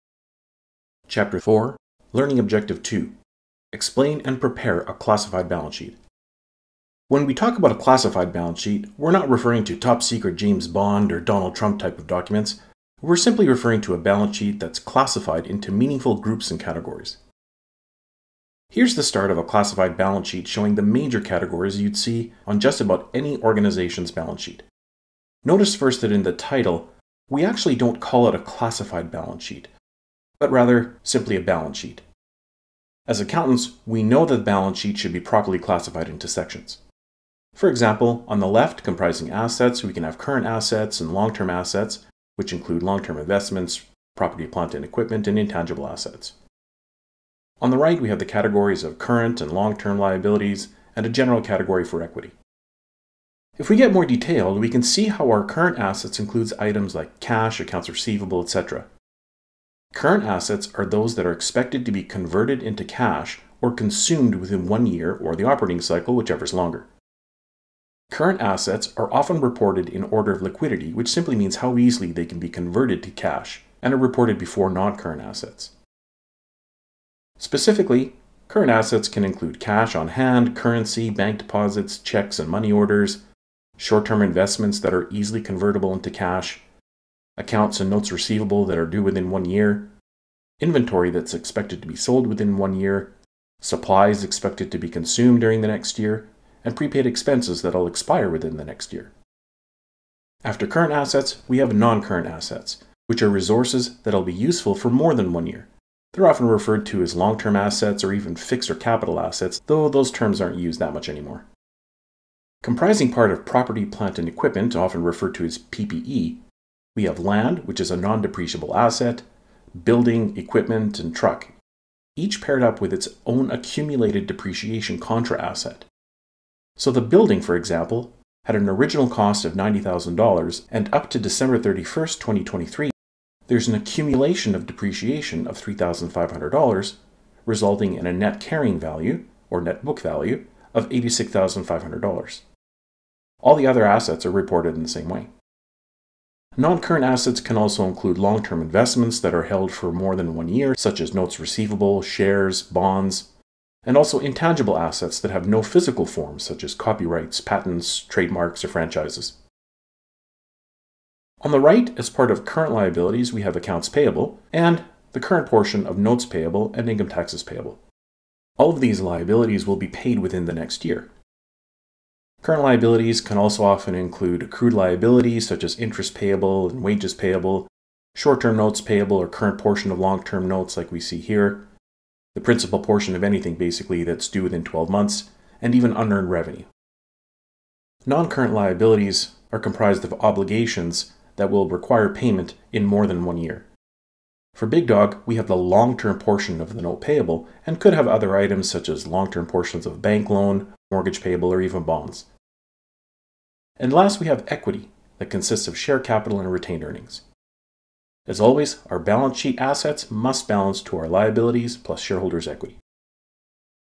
Learning Objective 4.2: The Classified Balance Sheet
Chapter 4: The Classified Balance Sheet and Related Disclosures
Learning Objective 2: Explain and prepare a classified balance sheet